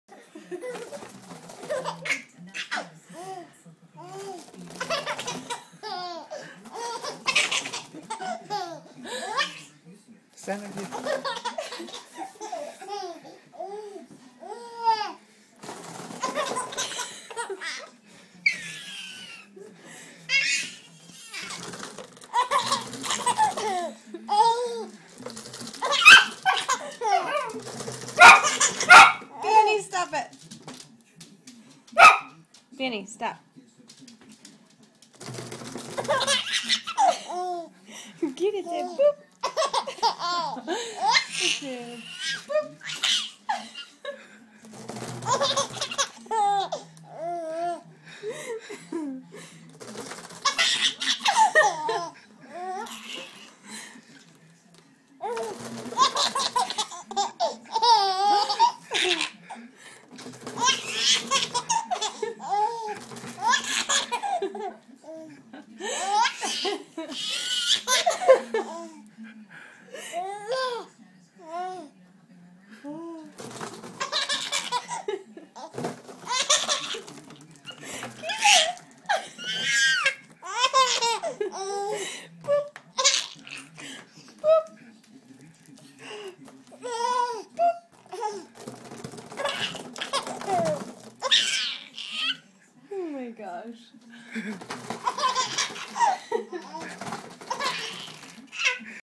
Baby laughing hysterically at a balloon!
Marley finds this balloon to be very funny